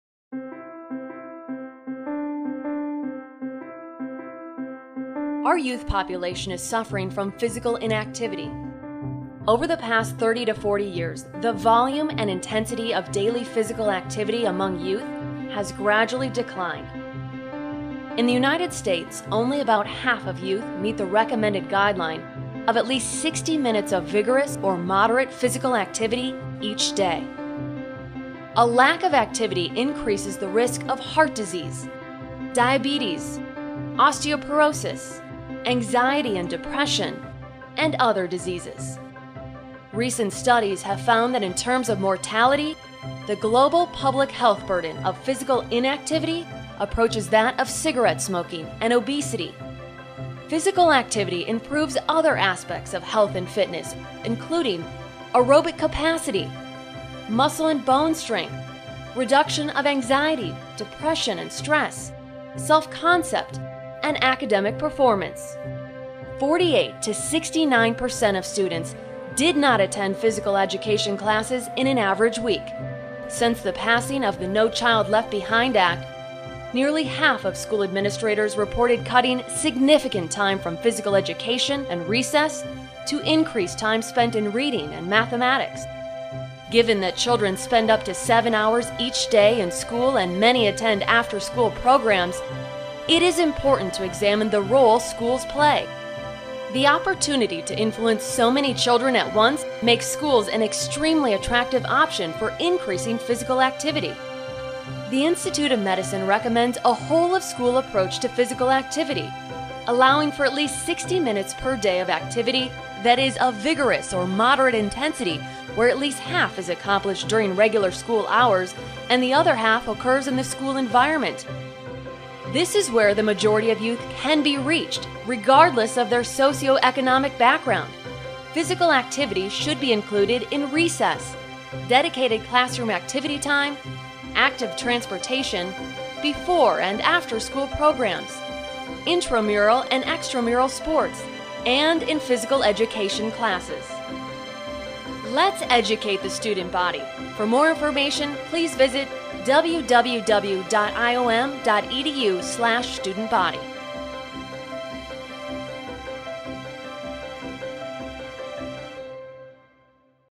Educating the Student Body: A Video Infographic with IOM
Our youth population is suffering from physical inactivity. We got to work with our friends at the Institute of Medicine to illustrate their latest report that examines the status of physical activity and physical education efforts in schools, how physical activity and fitness affect health outcomes, what can be done to help schools get students to become more active in the school environment. to learn more.